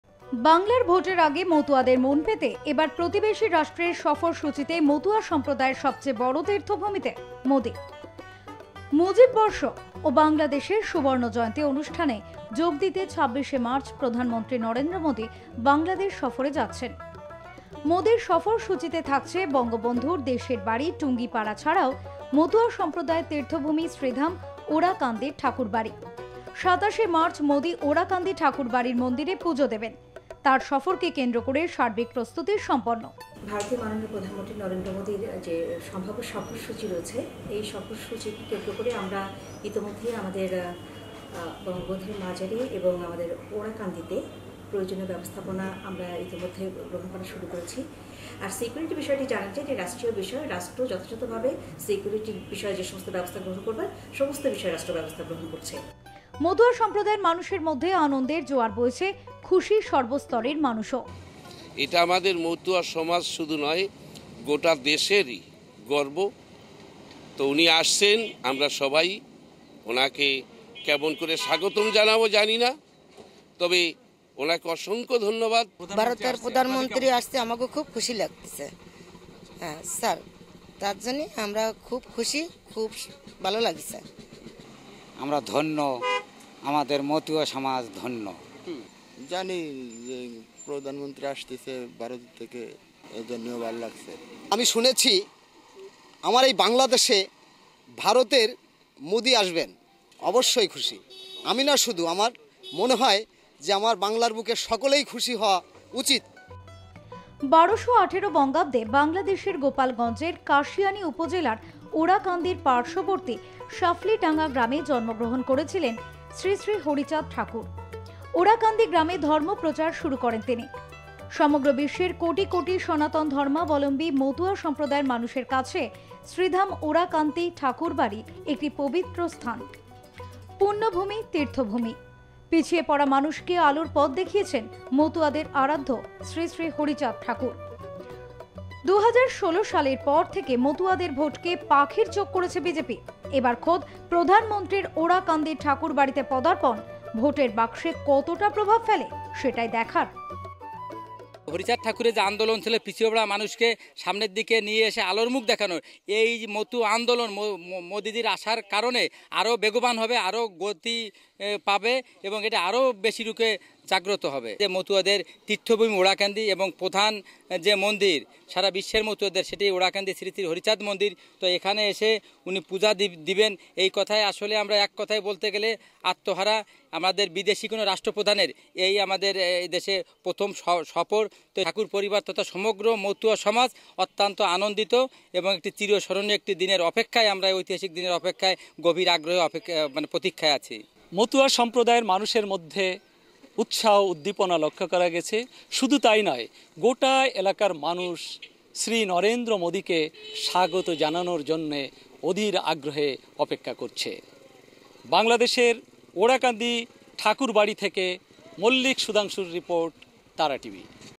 # Narandra _ Modi # Bangaladesh _ Orakandi # Matua _ Thakurbari #
বাংলার ভোটের আগে মতুয়াদের মন পেতে এবার প্রতিবেশি রাষ্ট্রের সফরসূচিতে মতুয়া সম্প্রদায়ের সবচেয়ে বড় তীর্থ ভূমিতে মোদি ।

মুজিববর্ষ ও বাংলাদেশের সুবর্ণ জয়ন্তী অনুষ্ঠানে যোগ দিতে ২৬ শে মার্চ প্রধানমন্ত্রী নরেন্দ্র মোদি বাংলাদেশ সফরে যাচ্ছেন । মোদির সফরসূচিতে থাকছে বঙ্গবন্ধুর দেশের বাড়ি টুঙ্গিপাড়া ছাড়াও মতুয়া সম্প্রদায়ের তীর্থভূমি শ্রীধাম ওড়াকান্দির ঠাকুর বাড়ি। ২৭ শে মার্চ মোদি ওড়াকান্দি ঠাকুর বাড়ির মন্দিরে পুজো দেবেন। তাঁর সফরকে কেন্দ্র করে সার্বিক প্রস্তুতি সম্পন্ন । মতুয়া সম্প্রদায়ের মানুষের মধ্যে আনন্দের জোয়ার বইছে। খুশি সর্বস্তরের মানুষও।

১২১৮ বঙ্গাব্দে বাংলাদেশের গোপালগঞ্জের কাশিয়ানি উপজেলার ওড়াকান্দির পার্শবর্তী সাফলিডাঙ্গা গ্রামে জন্মগ্রহণ করেছিলেন শ্রীশ্রী হরিচাদ ঠাকুর। ওরাকান্দি গ্রামে ধর্মপ্রচার শুরু করেন তিনি। সারা বিশ্বের কোটি কোটি সনাতন ধর্মালম্বী মতুয়া সম্প্রদায়ের মানুষের কাছে শ্রীধাম ওড়াকান্দি ঠাকুর বাড়ি একটি পবিত্র স্থান, পূণ্য ভুমি-তীর্থভুমি। পিছিয়ে পড়া মানুষকে আলোর পথ দেখিয়েছেন মতুয়াদের আরাধ্য শ্রীশ্রী হরিচাদ ঠাকুর । ২০১৬ সালের পর থেকে মতুয়াদের ভোটকে পাখির চোখ করেছে বিজেপি । এবার খোদ প্রধানমন্ত্রীর ওড়াকান্দির ঠাকুর বাড়িতে পদার্পণ ভোটের বাক্সে কতটা প্রভাব ফেলে সেটাই দেখার ।